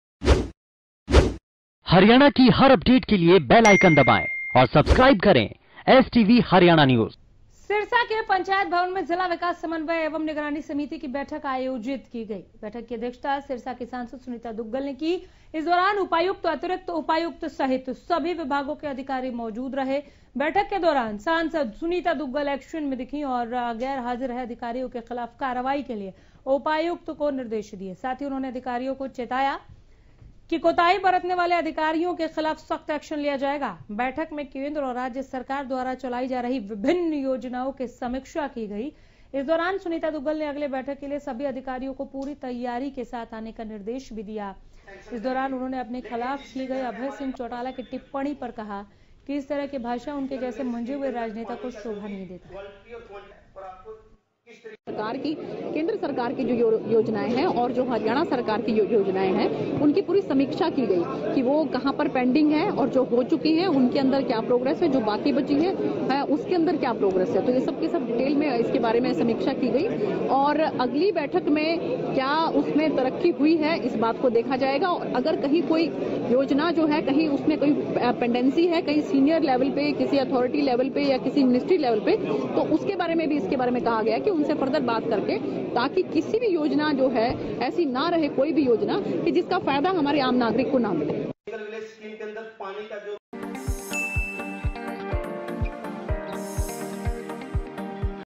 सिरसा: जिला विकास समन्वय एवं निगरानी समिति की बैठक.. सांसद सुनीता दुग्गल ने की बैठक की अध्यक्षता..
सिरसा के पंचायत भवन में जिला विकास समन्वय एवं निगरानी समिति की बैठक आयोजित की गई। बैठक की अध्यक्षता सिरसा की सांसद सुनीता दुग्गल ने की… इस दौरान उपायुक्त , अतिरिक्त उपायुक्त सहित सभी विभागों के अधिकारी भी मौजूद थे...बैठक के दौरान सांसद सुनीता दुग्गल एक्शन में दिखी और गैर हाजिर रहे अधिकारियों के खिलाफ कार्रवाई के लिए उपायुक्त को निर्देश दिए। साथ ही उन्होंने अधिकारियों को चेतावनी देते हुए कहा कि कोताही बरतने वाले अधिकारियों के खिलाफ सख्त एक्शन लिया जाएगा। बैठक में केंद्र और राज्य सरकार द्वारा चलाई जा रही विभिन्न योजनाओं की समीक्षा की गई इस दौरान सुनीता दुग्गल ने अगले बैठक के लिए सभी अधिकारियों को पूरी तैयारी के साथ आने का निर्देश भी दिया...इस दौरान उन्होंने अपने खिलाफ की गई अभय सिंह चौटाला की टिप्पणी पर कहा कि… इस तरह की भाषा उनके जैसे मंझे हुए राजनेता को शोभा नहीं देता है…